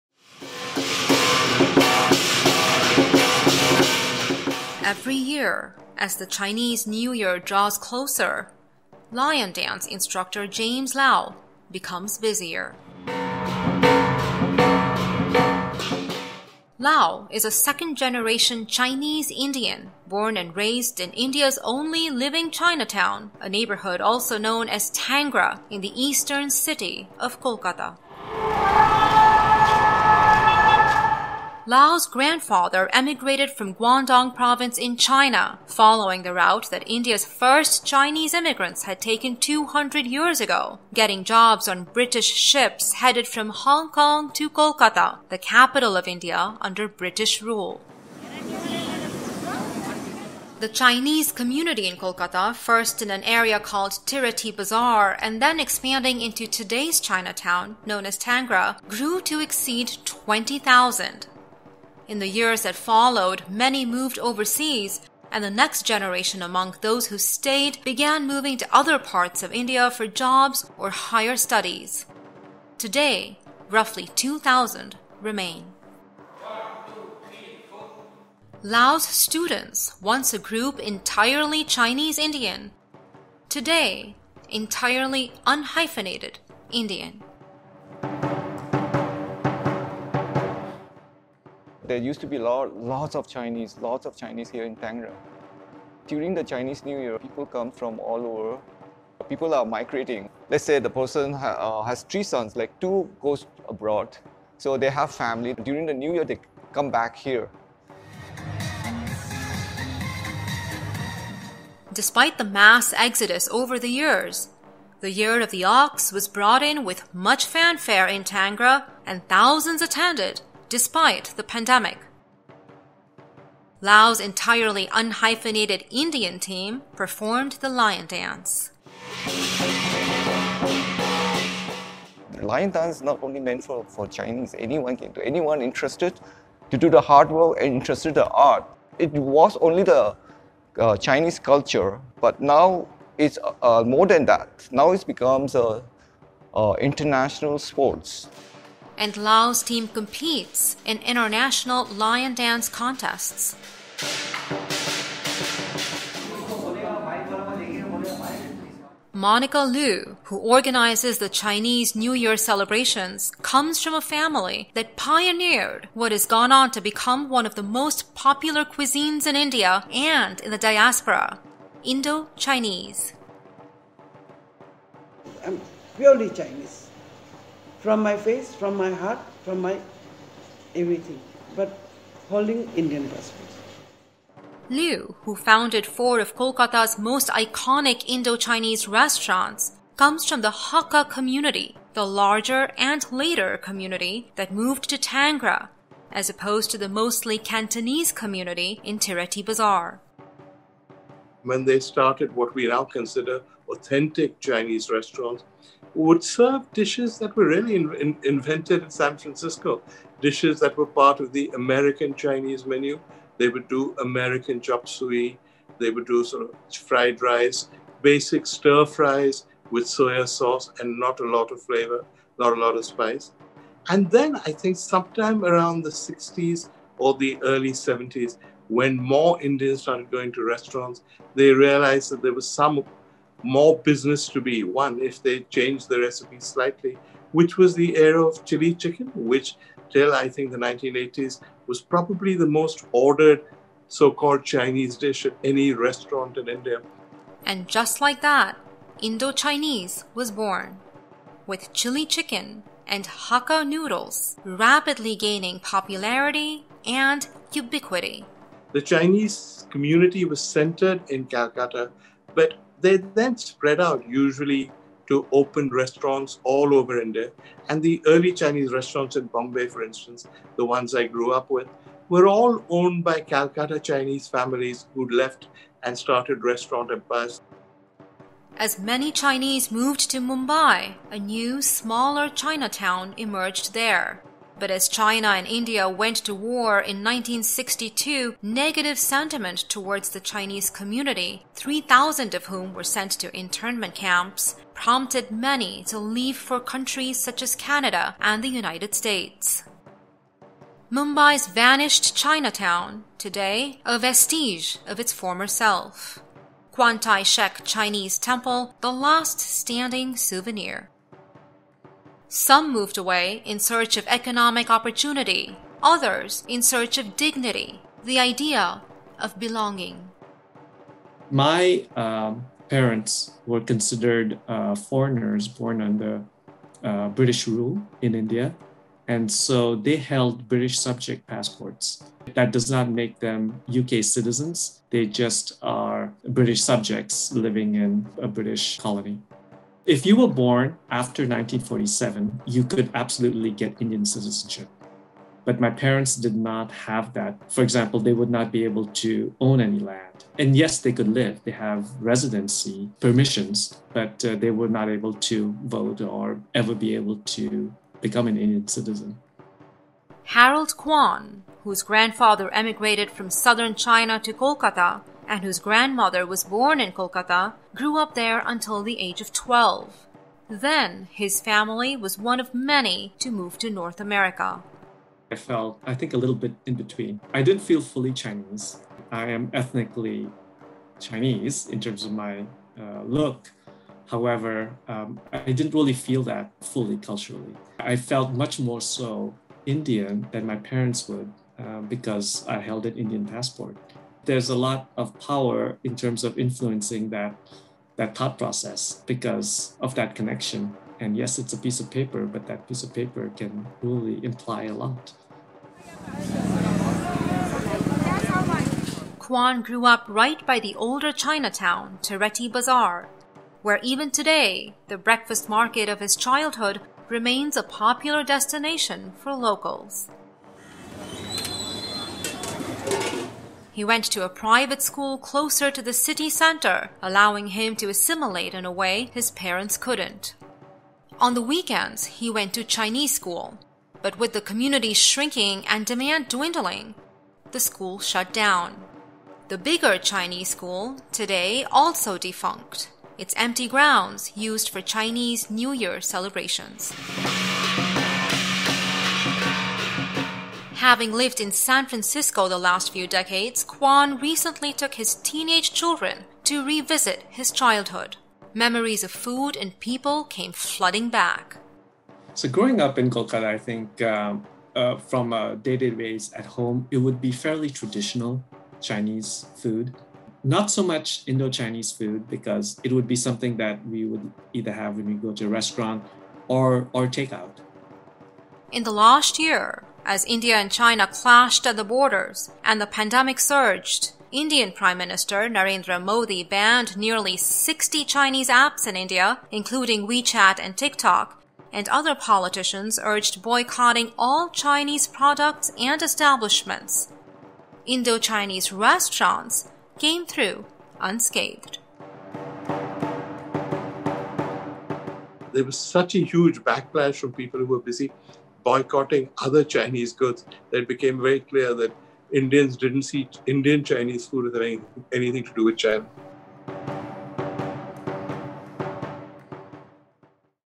India's Chinese Community Fading But Not Forgotten
Kolkata, the eastern Indian city that was the capital under British rule and served as the port of entry for much of the world, is a mecca of melting pots and dimming histories. The city has long been a stronghold for the country's Jews, Anglo-Indians, Armenians, Afghans, Tibetans and Iraqis.
Most communities are slowly fading. This is a piece about one such people - the Indian-Chinese.